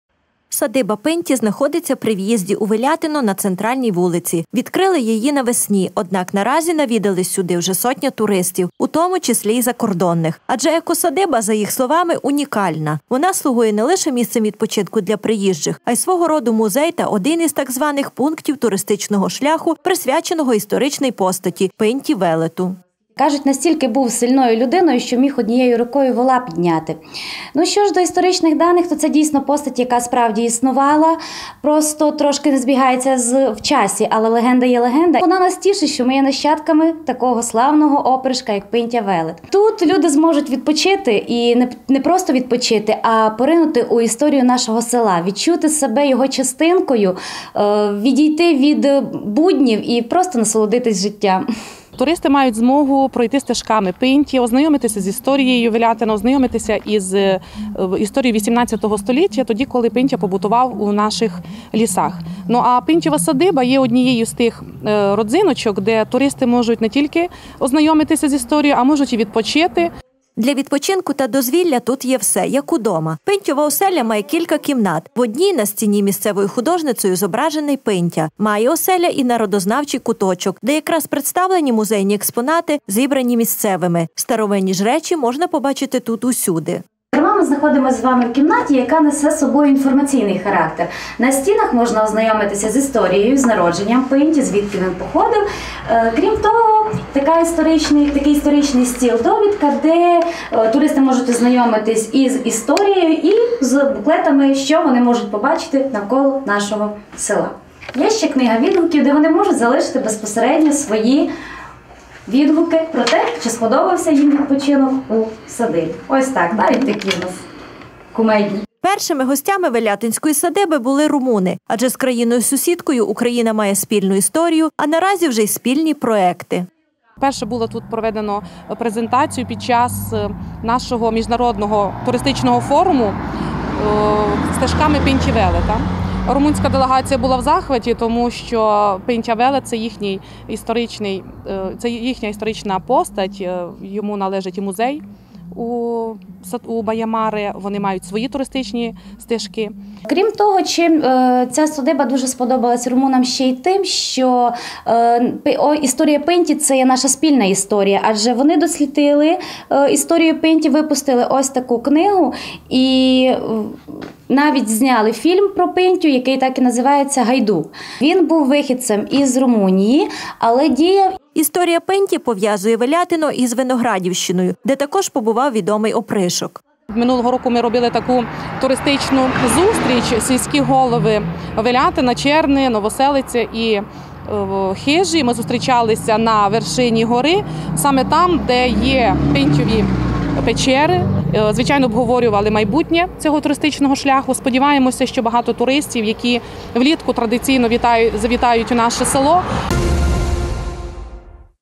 «Пинтьова оселя»
Садиба Пинті знаходиться при в’їзді у Велятино, на центральній вулиці. Відкрили її навесні, однак наразі навідалась сюди вже сотня туристів, у тому числі й закордонних. Адже екосадиба, за їх словами, унікальна. Вона слугує не лише місцем відпочинку для приїжджих, а й свого роду музей та один із так званих пунктів туристичного шляху присвяченого історичній постаті - Пинті Велету. Для відпочинку та дозвілля тут є все, як удома. «Пинтьова оселя» має кілька кімнат. В одній на стіні місцевою художницею зображений Пинтя. Має оселя й народознавчий куточок, де якраз представлені музейні експонати, зібрані місцевими. Старовинні ж речі можна побачити тут усюди. Першими гостями Велятинської садиби були румуни, адже з країною – сусідкою Україна має спільну історію, а наразі вже й спільні проекти. Історія Пинті пов’язує Велятино і з Виноградівщиною, де також побував відомий опришок.